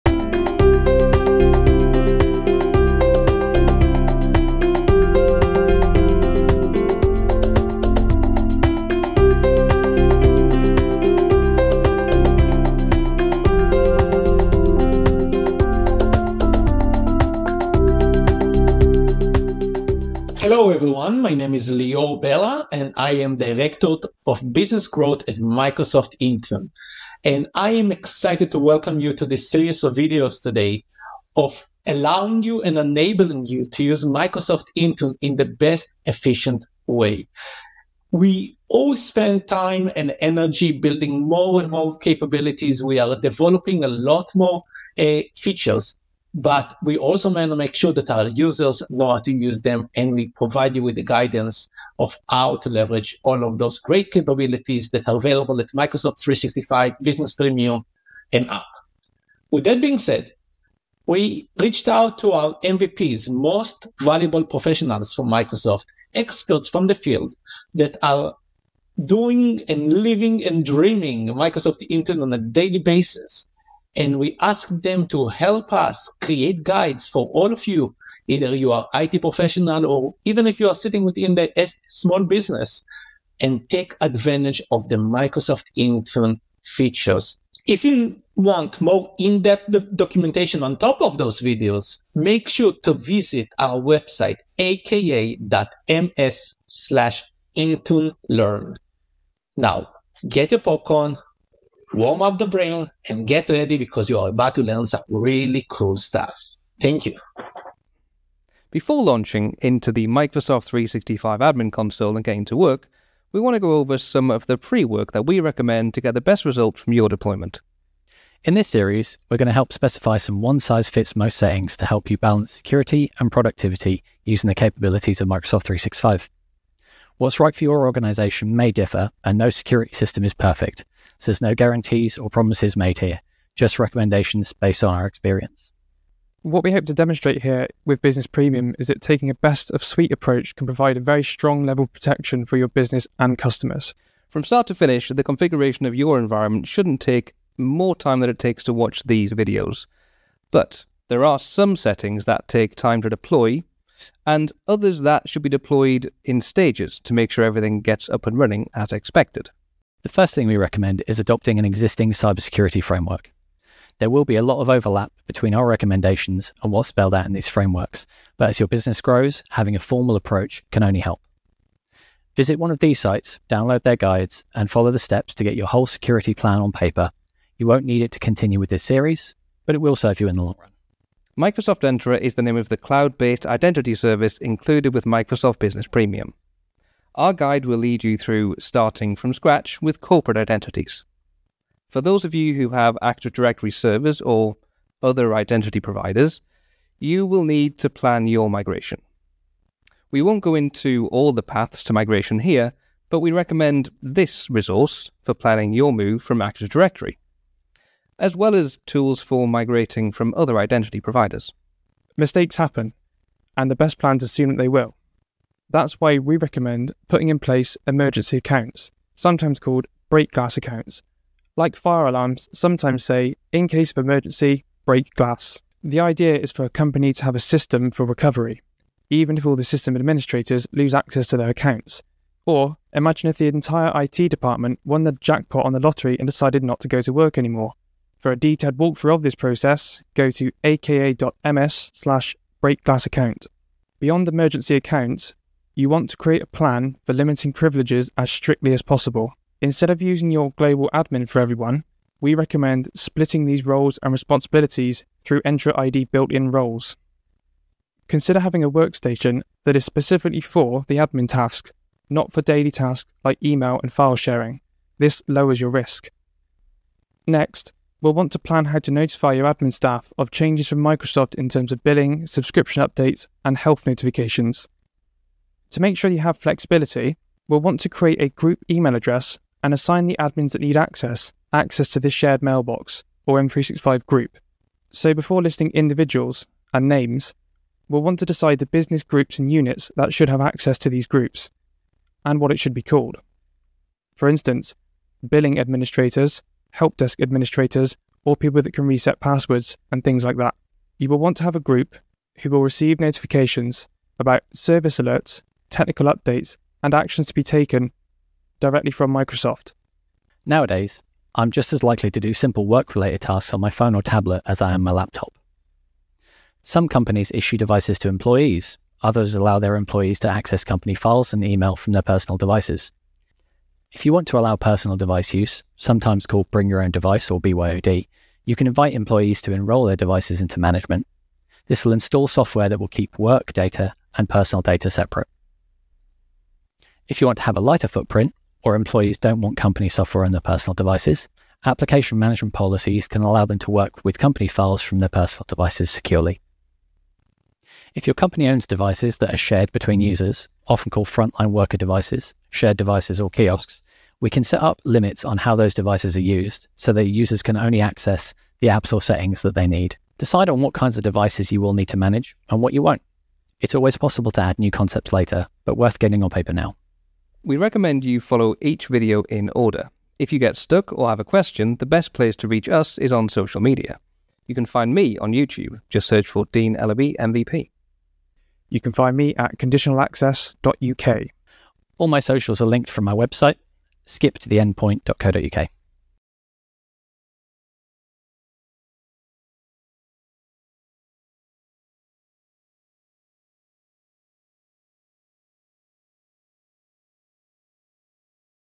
Achieve greater security and productivity with Microsoft Intune and Microsoft 365: Before You Begin
Learn some considerations and 'paper and pencil’ planning you should do before getting started with the rest of the videos in this series.

For more how-to guidance on getting started with Microsoft Intune,